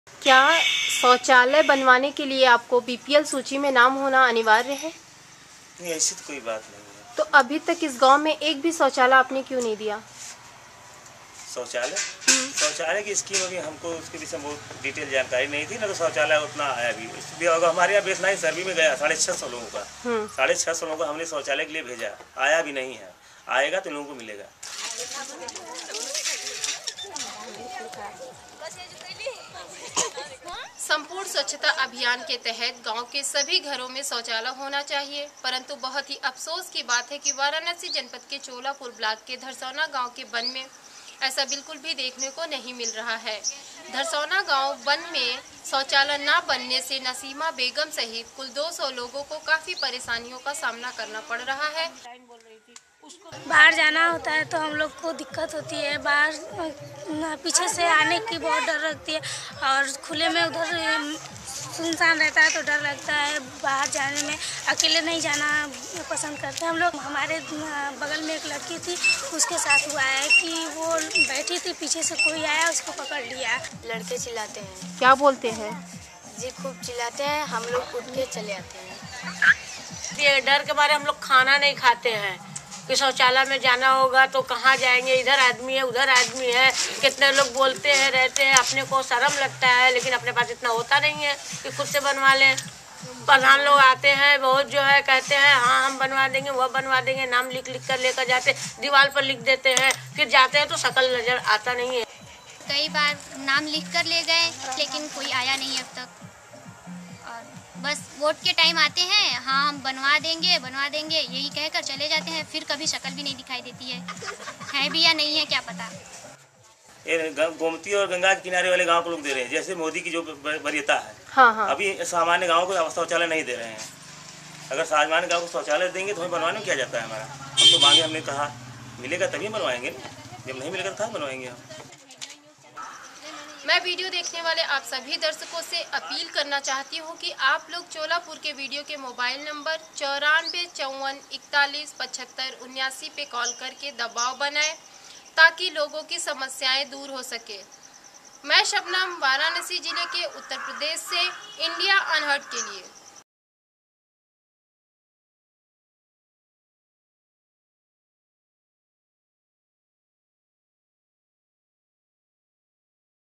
200 families need toilet
Community Correspondent Shabnam Begum reports from Varanasi, Uttar Pradesh for Video Volunteers.